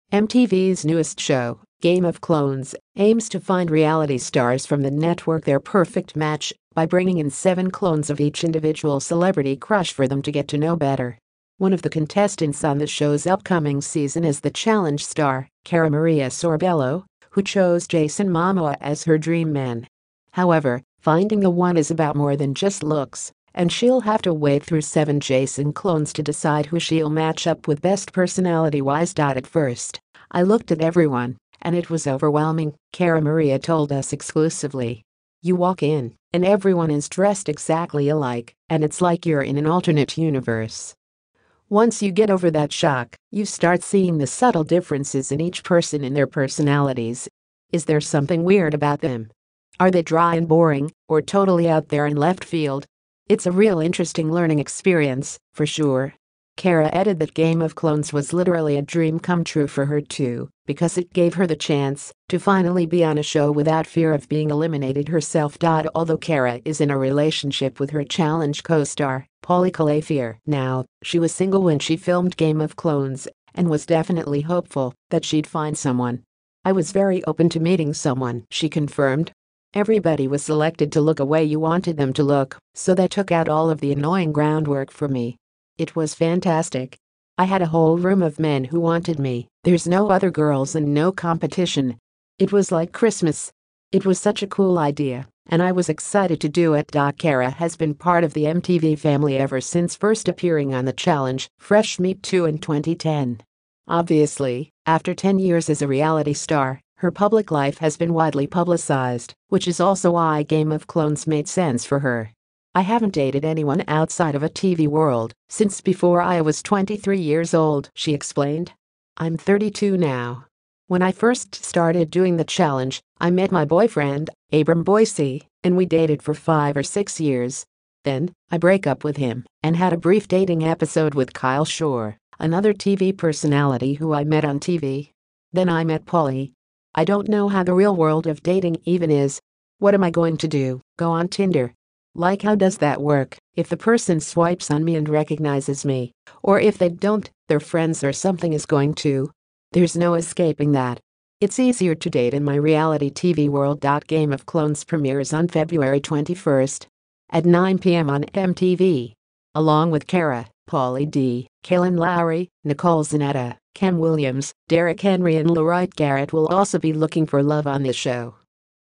'Game Of Clones': Cara Maria Reveals Why She's Searching For Love On MTV's New Dating Show
Cara Maria is one of seven MTV stars hoping to find their match on the network's new dating show, 'Game of Clones,' and we caught up with her EXCLUSIVELY about what she was looking for in her dream man and much more!